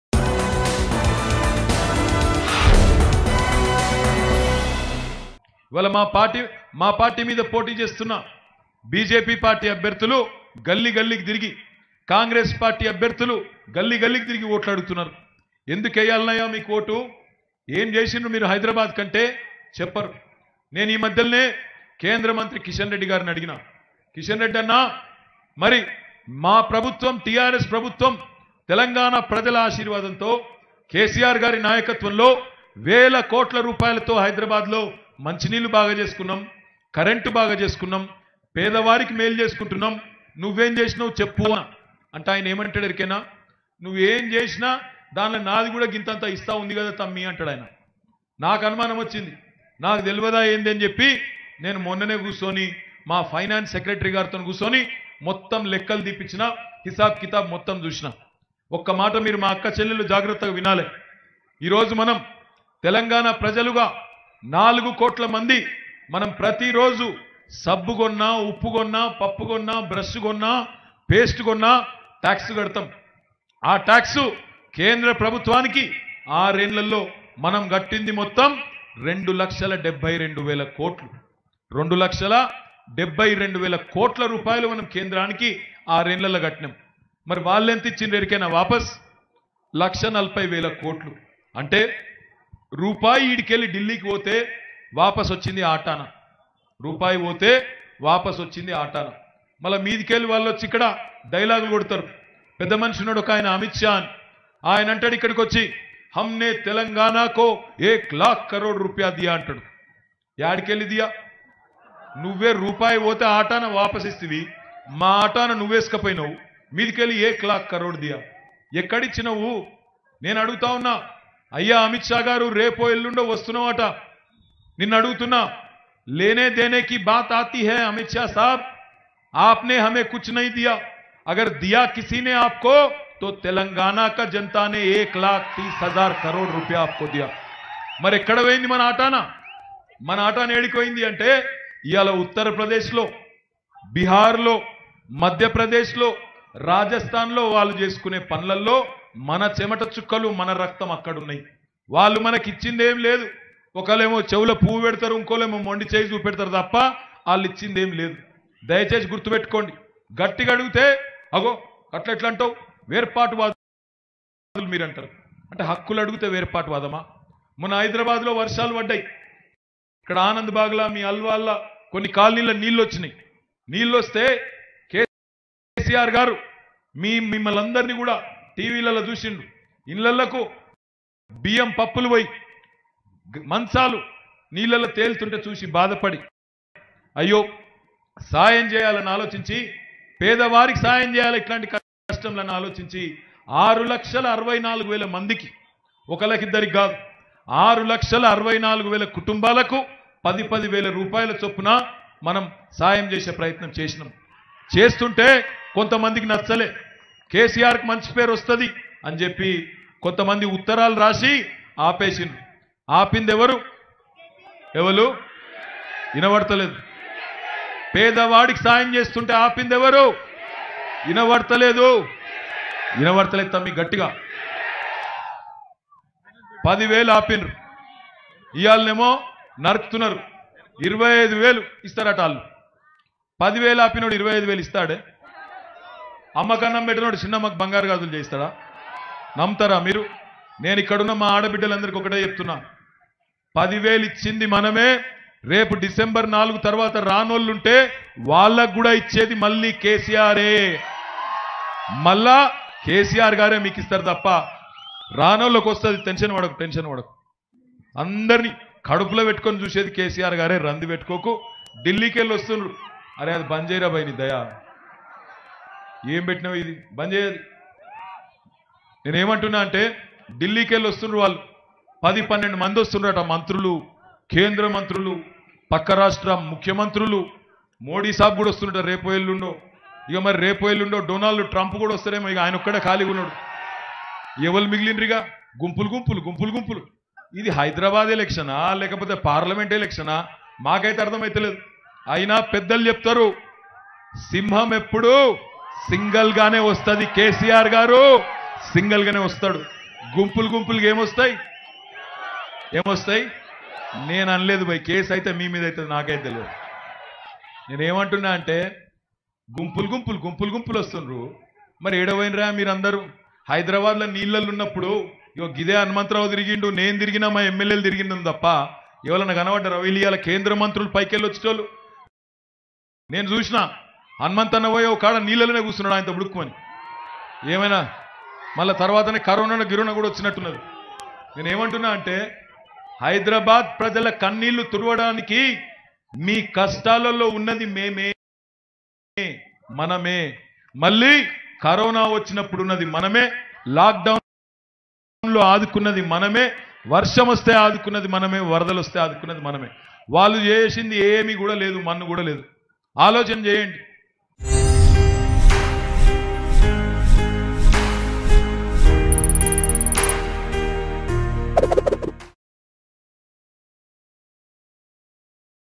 Minister KTR Praises CM KCR In GHMC Election Campaign | Telangana Political News | Mango News
Minister KTR Praises CM KCR In GHMC Election Campaign.